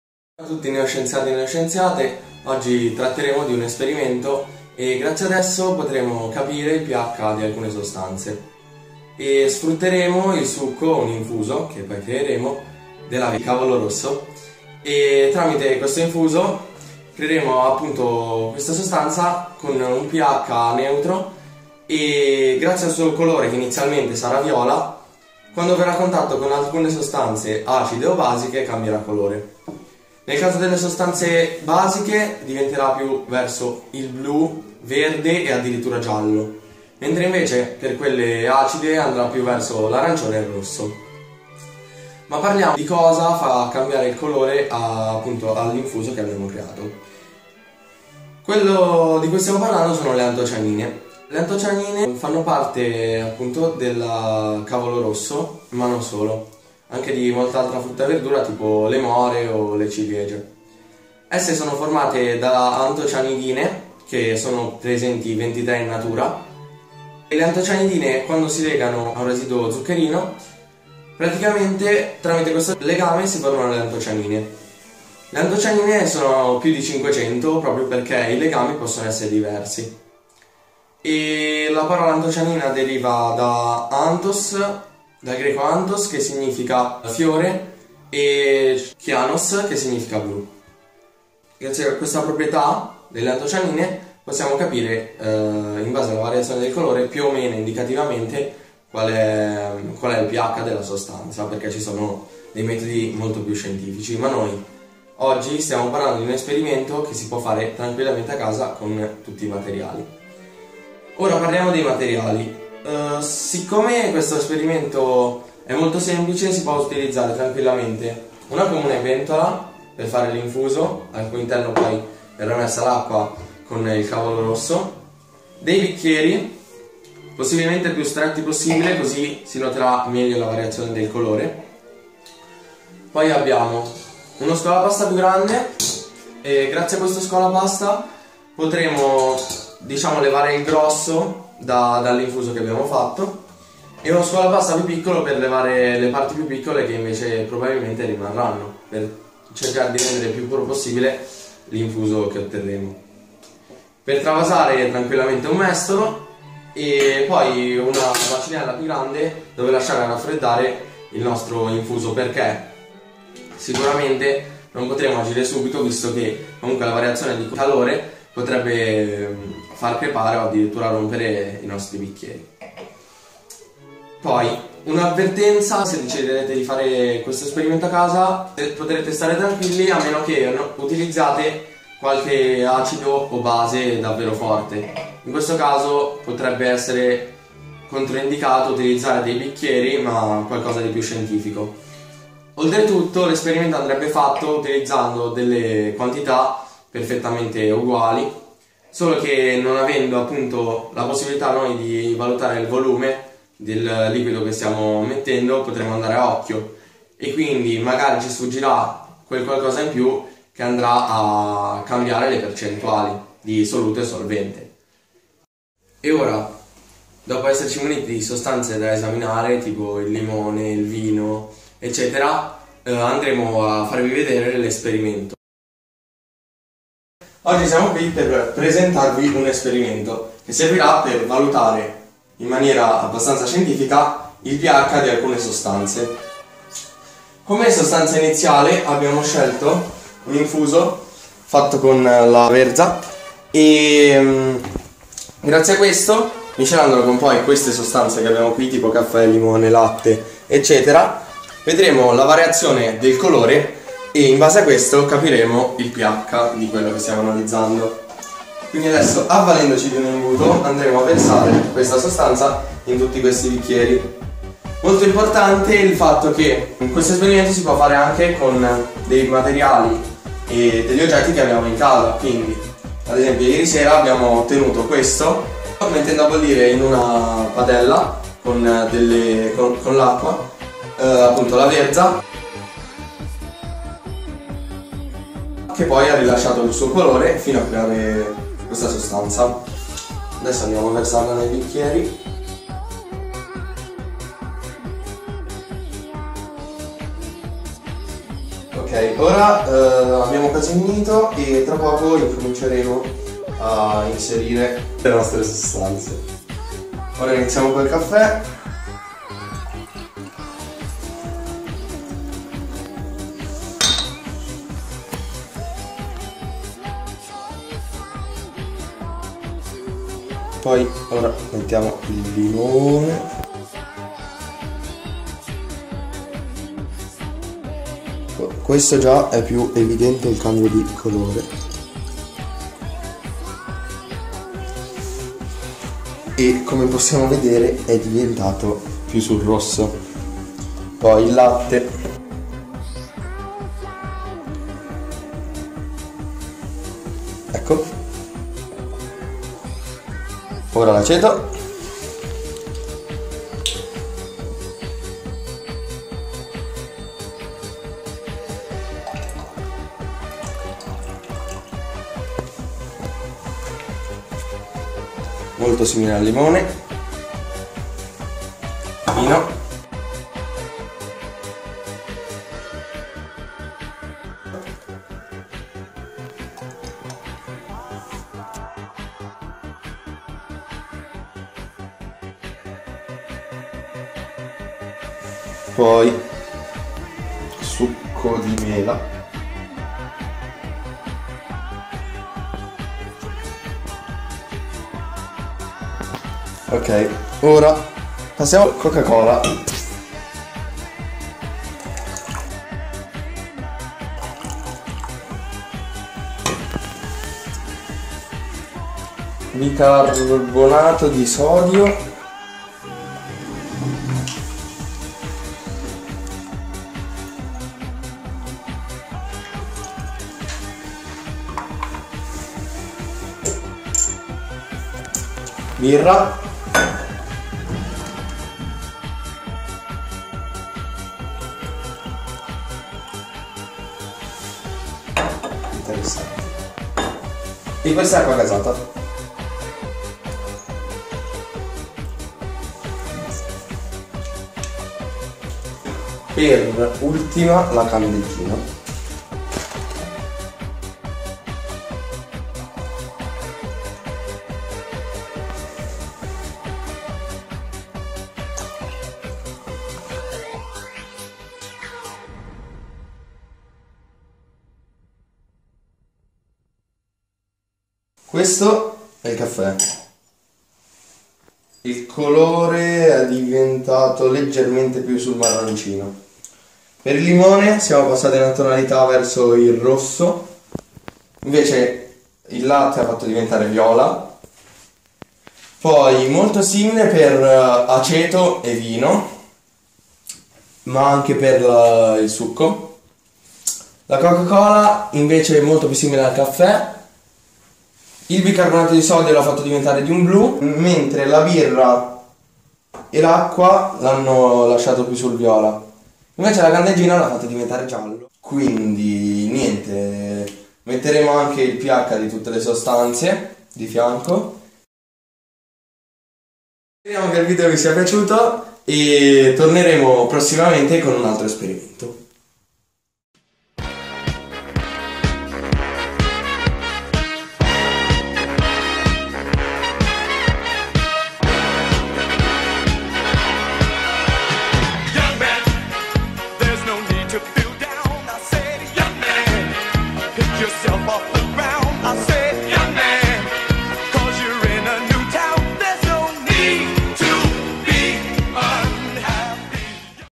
Esperimento Cavolo Rosso
Esperimento sull'utilizzo del cavolo rosso come indicatore del ph